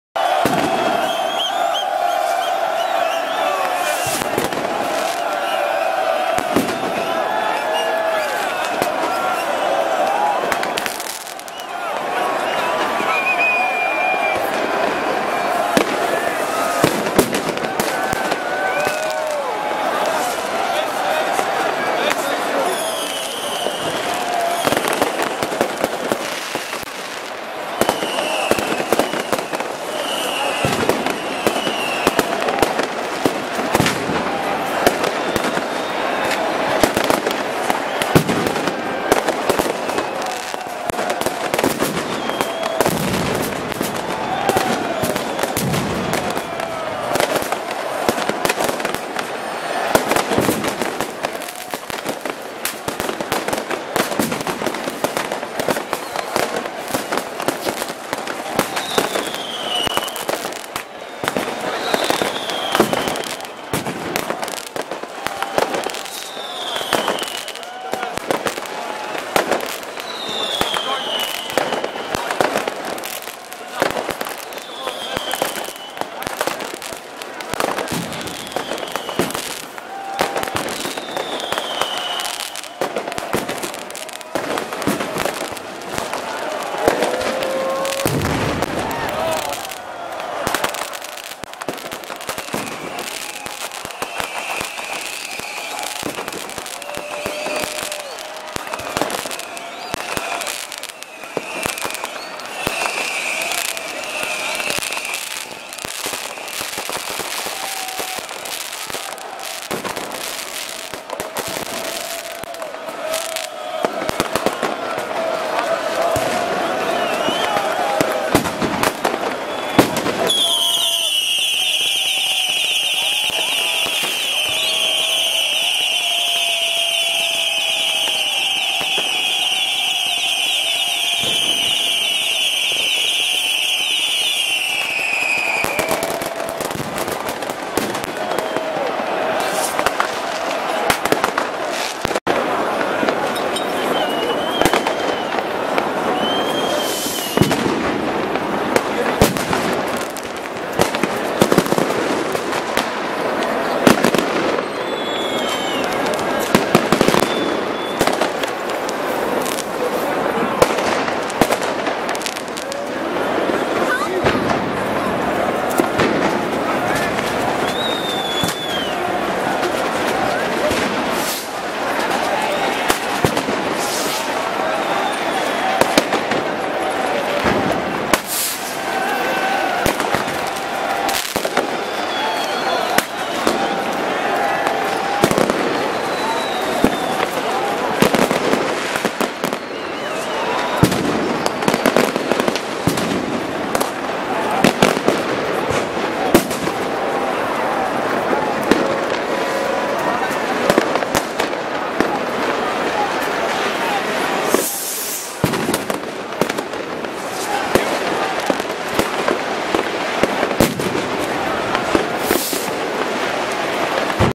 Amsterdam FIREWORKS New Years Eve 2016
with the square in front of the Royal Palace packed with New Year's Eve revelers for the 2016 celebration, fireworks erupts everywhere to celebrate the New Year.